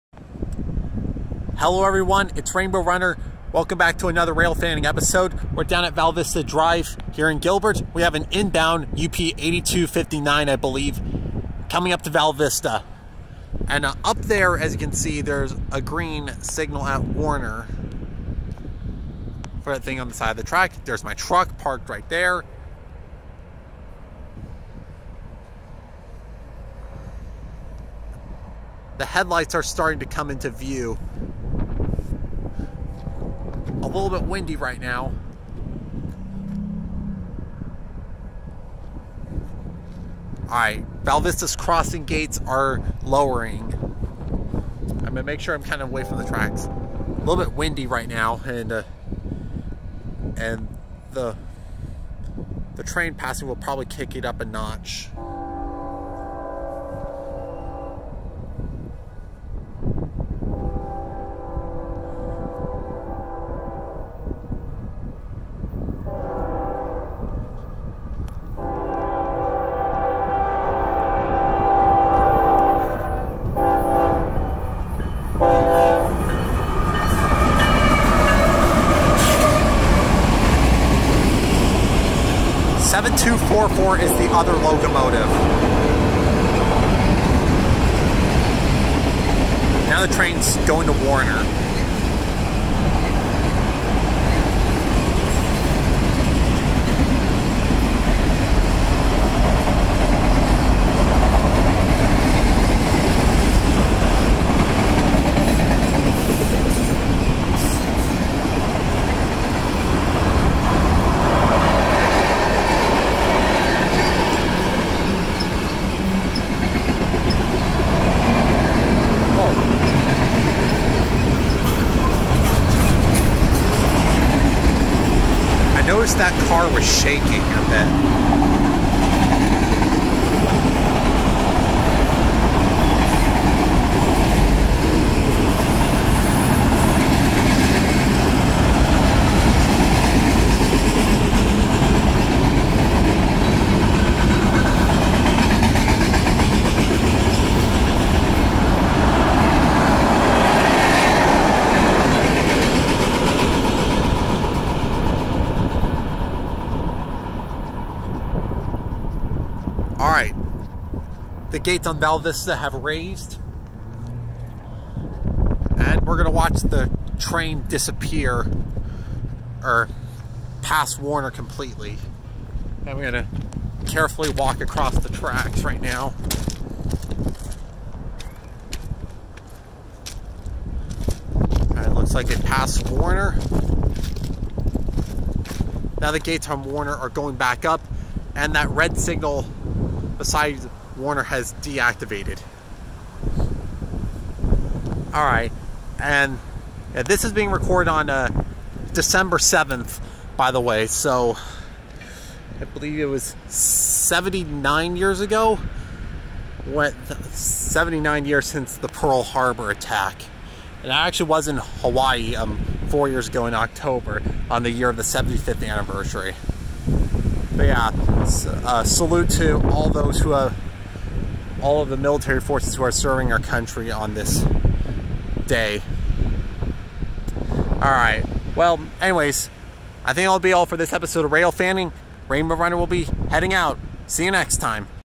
Railfanning #66: UP 8259 Inbound Manifest Train at Western Skies Golf Course
(12/7/2020) Austyn once again informed me of this train, but it was stopped at Germann for a while. I was a bit concerned about missing it because of a car crash at Val Vista and Ray. Plus I stopped at the Circle K there to not only get gas, but use the plaza to go around the accident.

Anyways, I parked my truck by the car wash in the Target plaza where I could easily walk across the tracks to the field where the golf course is in the Western Skies area. This was where I could see Val Vista's crossing.

UP 8259 led this Manifest Train, with UP 7244 coupled behind it. The engineer was able to horn show for me. I also noticed the red box car was shaking a bit as it was passing.

This was filmed on December 7, commemorating 79 years since the Pearl Harbor attack in 1941, the day that will live in infamy. I got to visit the USS Arizona Memorial when I was in Hawaii in October 2016 (that December was the 75th anniversary). A good tribute to all of the Armed Forces serving our country.